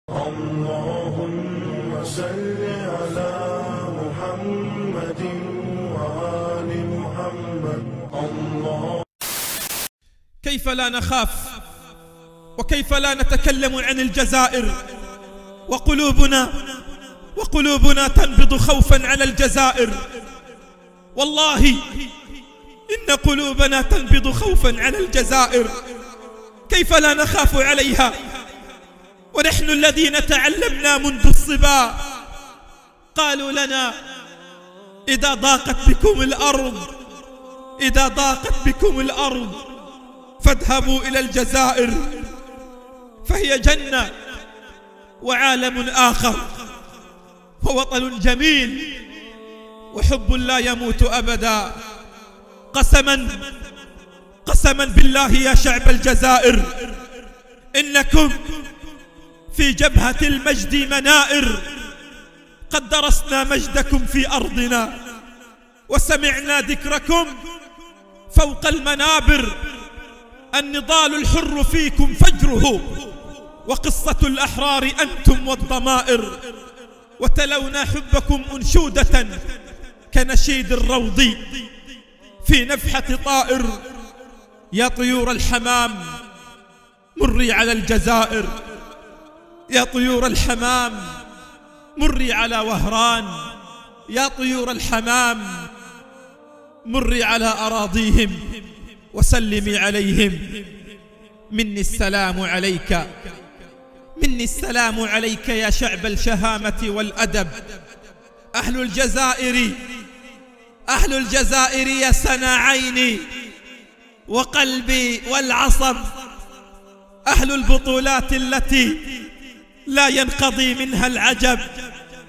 | Al-Hassanat | Un message du prédicateur palestinien au peuple algérien, (VOSTFR).
▶"Les héros qui ne terminent pas d’émerveiller."
▶ Imam Mahmoud Hassanat.
▶ Vidéo traduite et monté sur la chaîne.